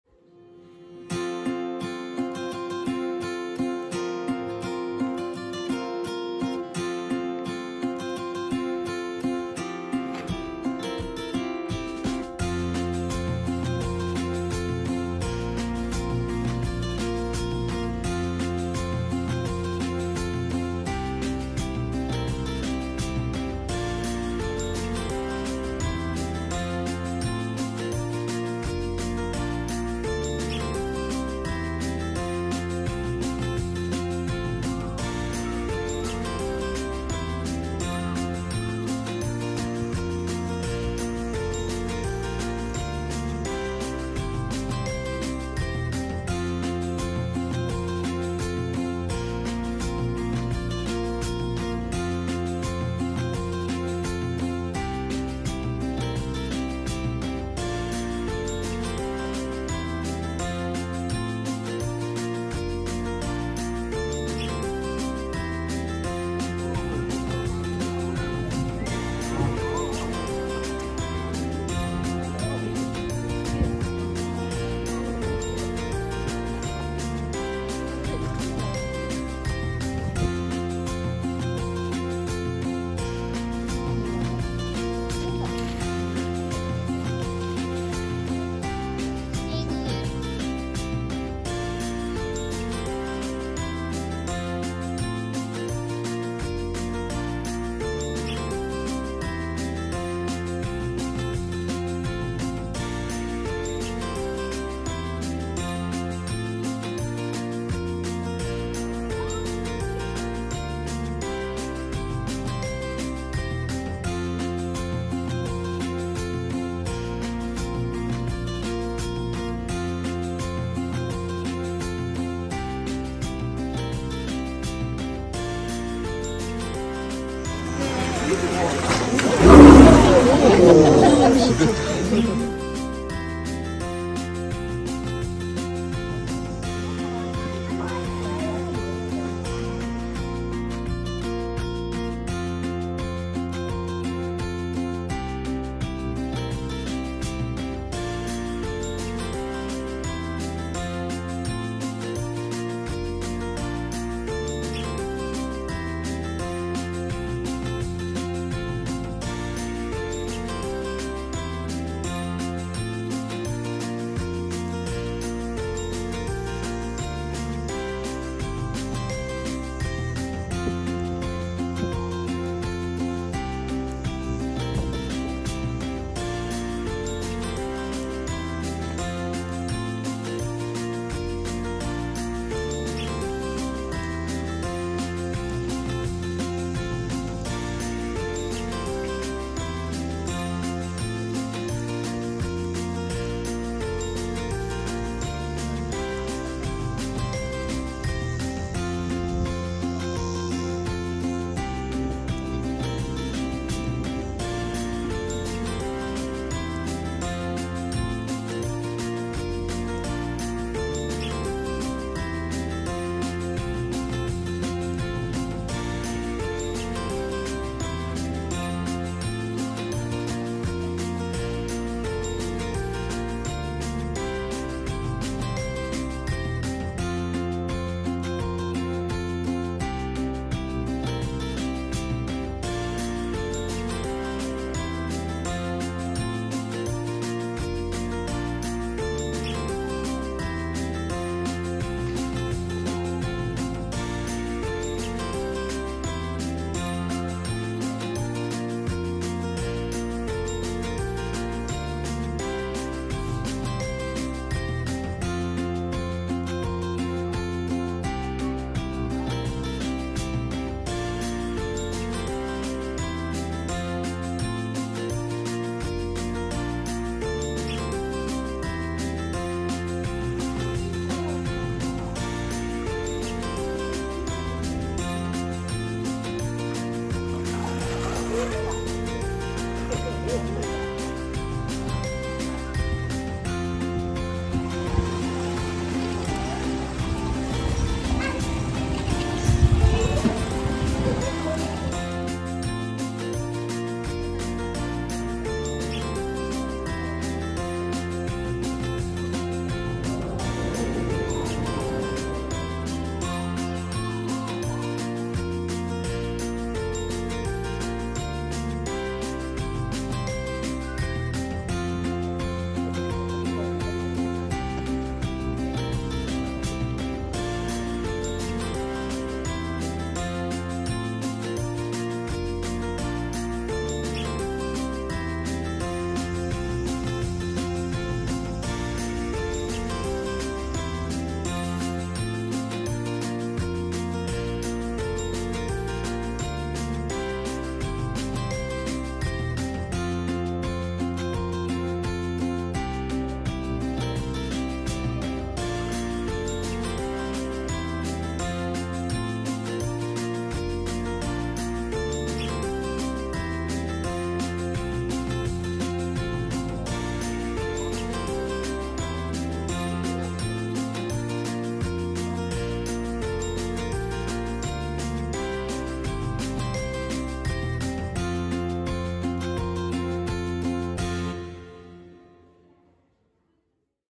20240815:今日の円山動物園:ちょびっと
2024年8月15日昼頃までの札幌市:円山動物園の動物達の様子です。※8月15日はライオン:パーチェの誕生日でした※